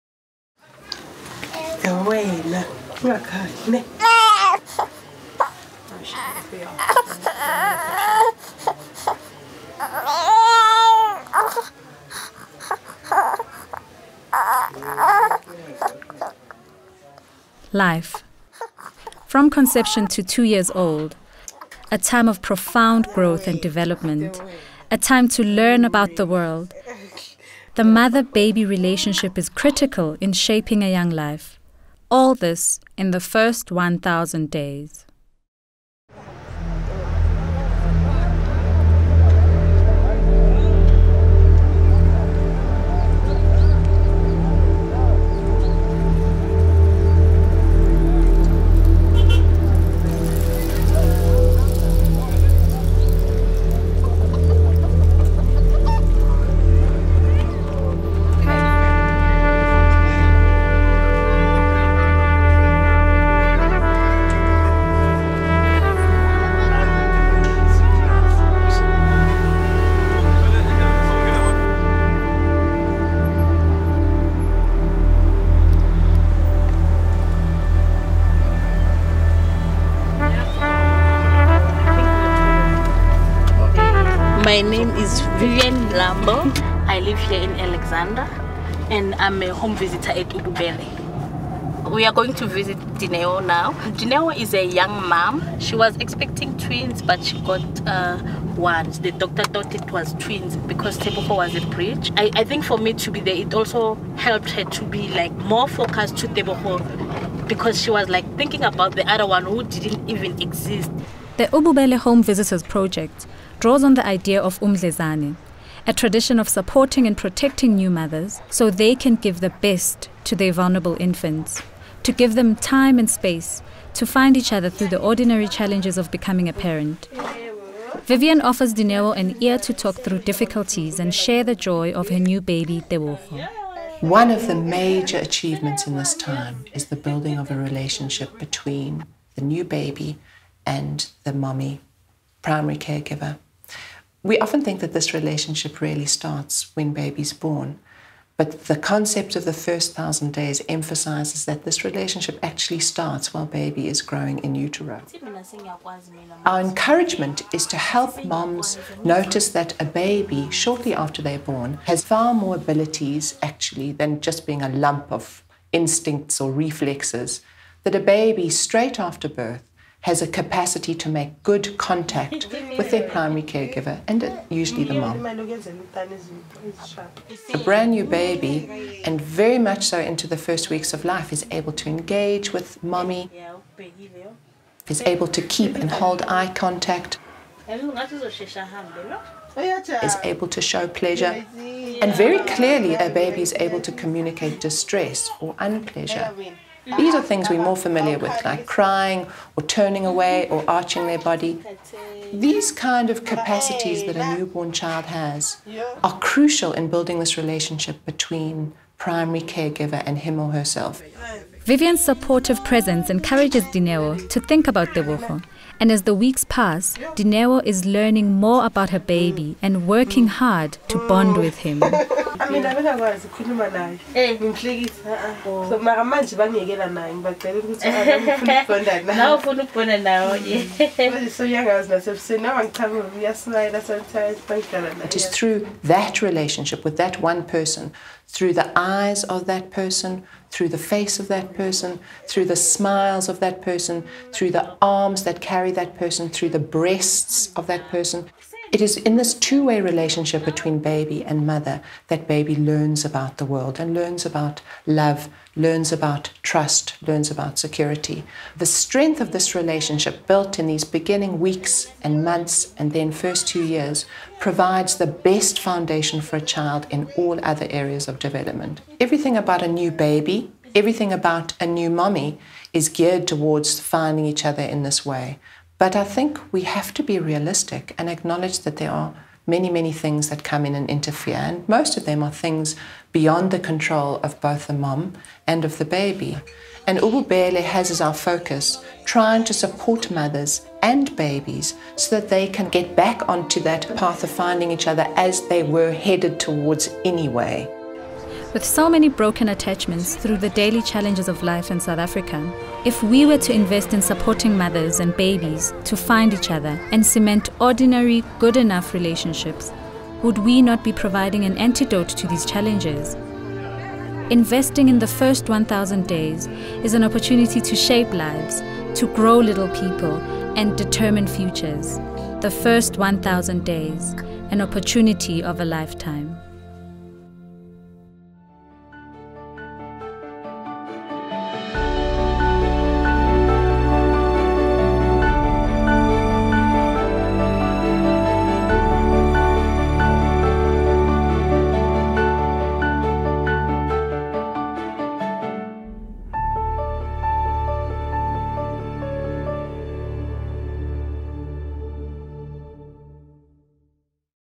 Umdlezane The First 1000 Days
An inspiring story of the love between a mother and her baby in Alexandra, Johannesburg, showing the importance of the First 1000 days of a child's life and how Ububele supports this bonding process.